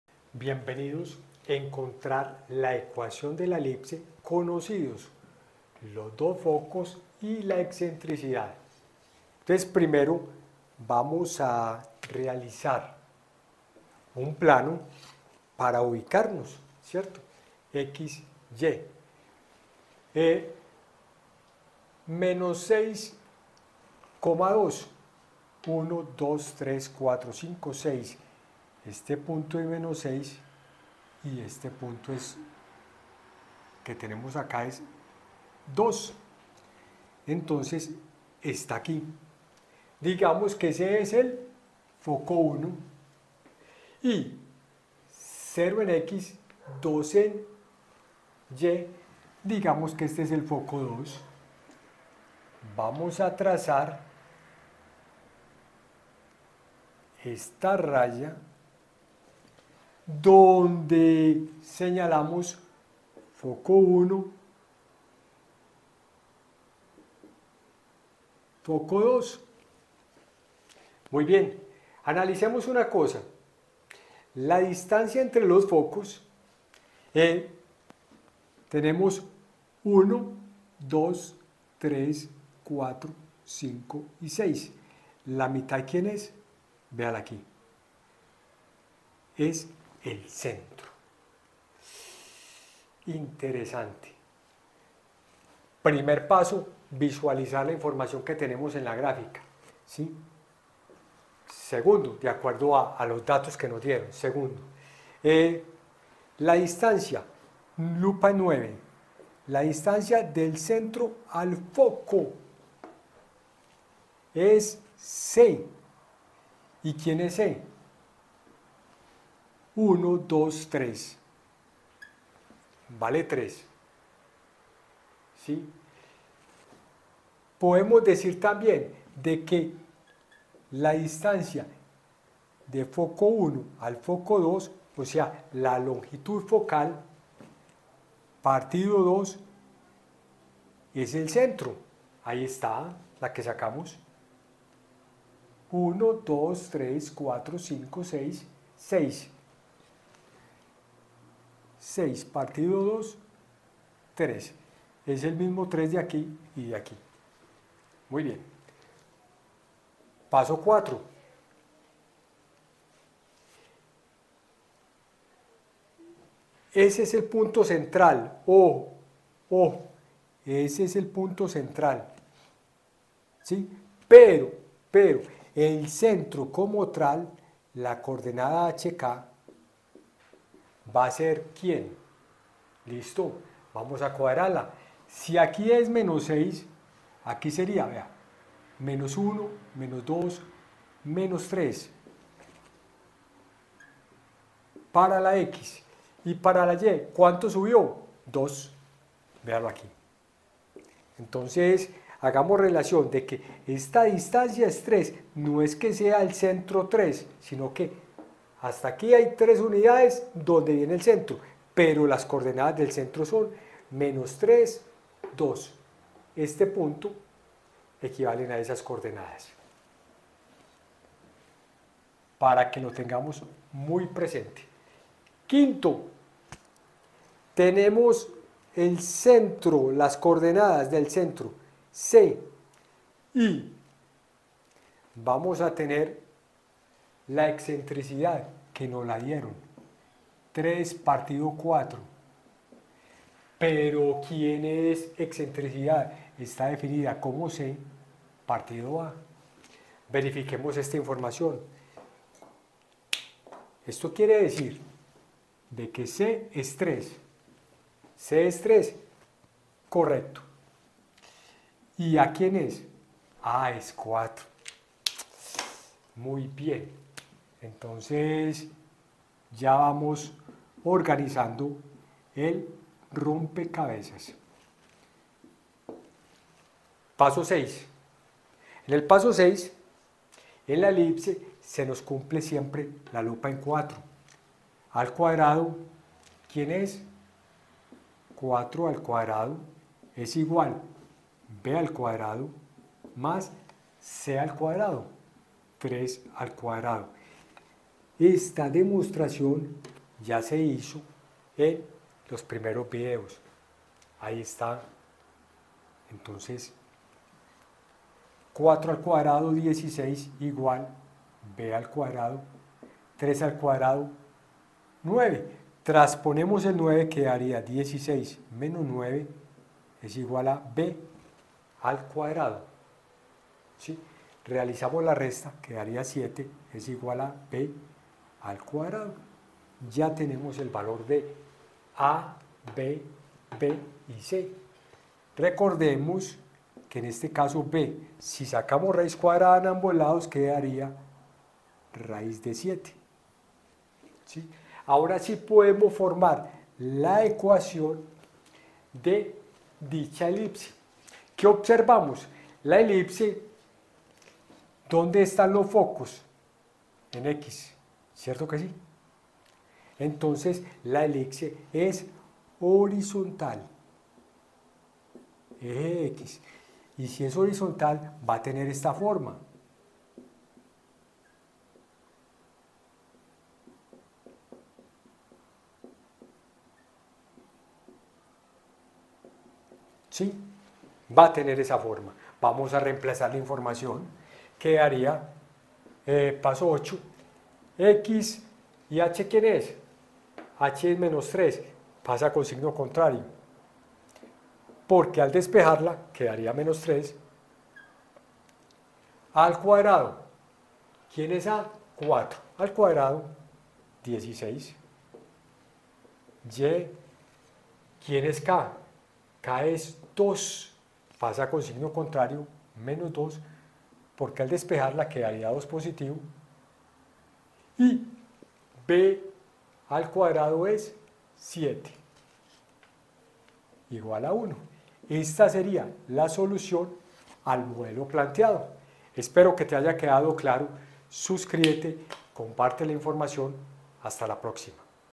Hallar la ecuación de la elipse dados los focos y la excentricidad.
profenini te enseña a encontrar la ecuación de la elipse dados los focos y la excentricidad.